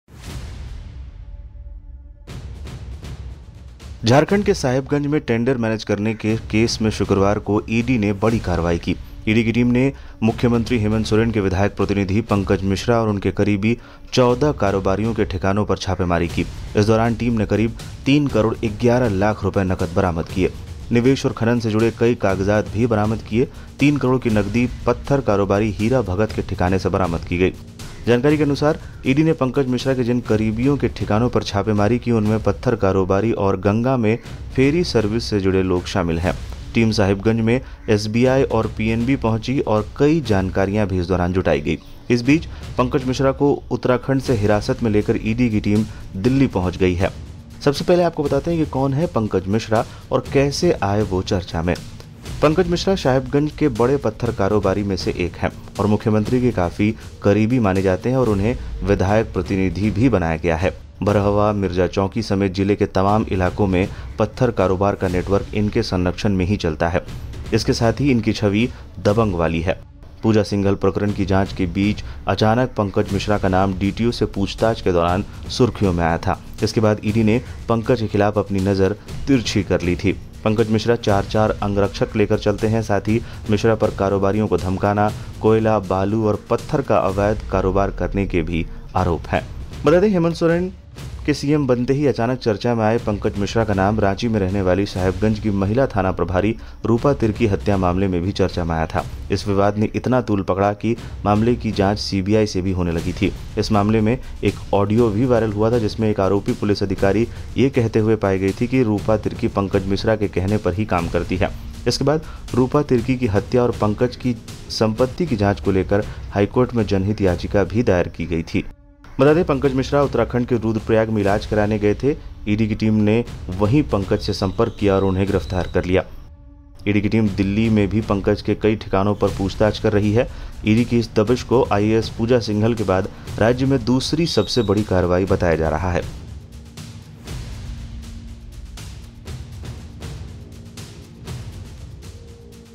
Jharkhand News: CM Hemant Soren के करीबी Pankaj Mishra ईडी की हिरासत में, जानिए कौन हैं
साहिबगंज में टेंडर मैनेज करने के केस में शुक्रवार को ईडी ने बड़ी कार्रवाई की। ईडी की टीम ने मुख्यमंत्री के विधायक प्रतिनिधि पंकज मिश्रा और उनके करीबी 14 कारोबारियों के ठिकानों पर छापेमारी की। इस दौरान दौरान टीम ने करीब 3 करोड़ 11 लाख नगद बरामद किए हैं। निवेश और खनन से जुड़े कई कागजात भी जब्त किए गए हैं। तीन करोड़ की नकदी पत्थर कारोबारी हीरा भगत के ठिकाने से बरामद की गई है।